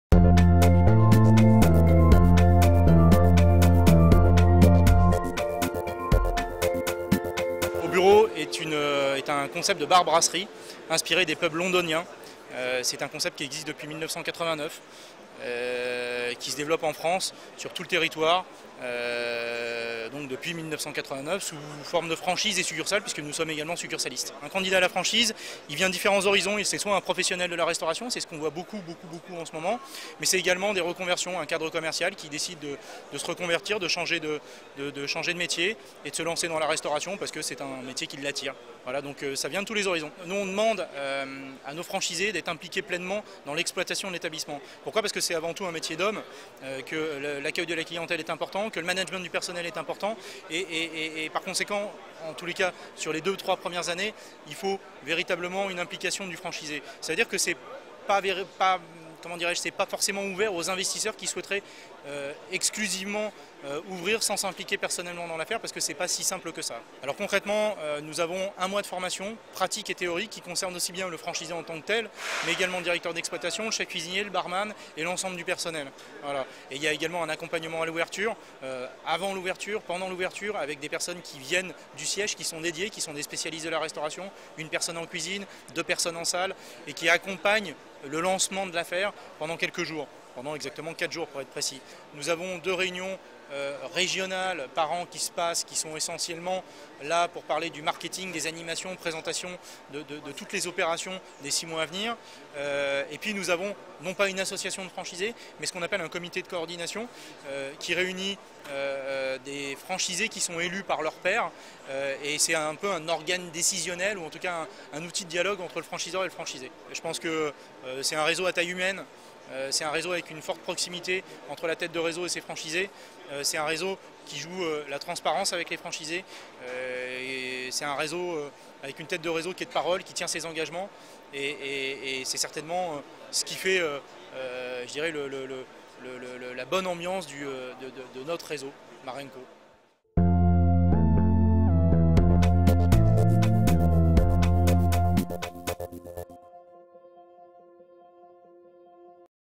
Au Bureau : La Franchise, interview du directeur Bars&Co. AC Franchise
La franchise Au Bureau est un bar-brasserie à thème qui vous offre 7 jours sur 7, midi et soir une restauration de qualité. Elle fait partie du groupe Bars&Co et compte près de 100 établissements en France.